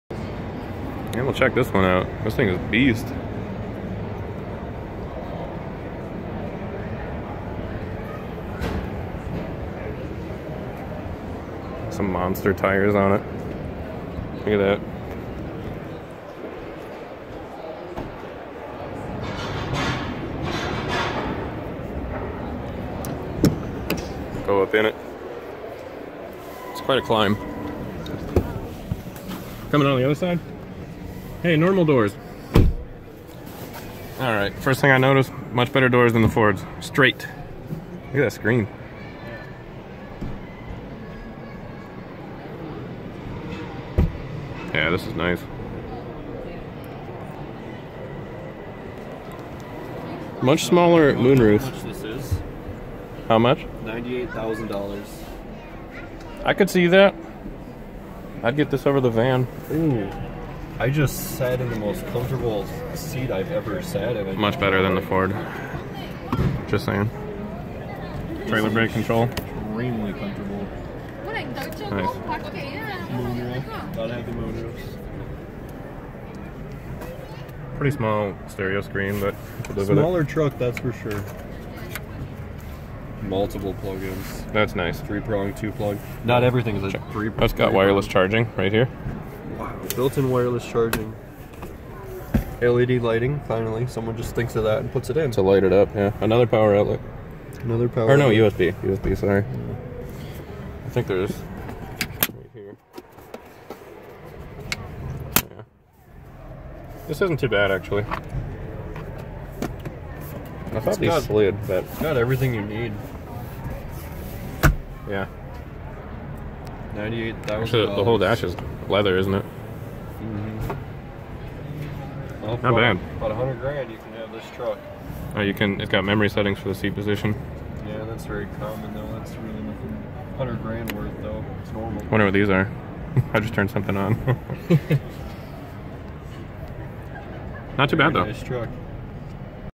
ONE HUNDRED THOUSAND DOLLARS for a 2019 GMC with a Duramax diesel??
Walk around tour and in depth look of a brand new custom built GMC Denali HD pickup truck!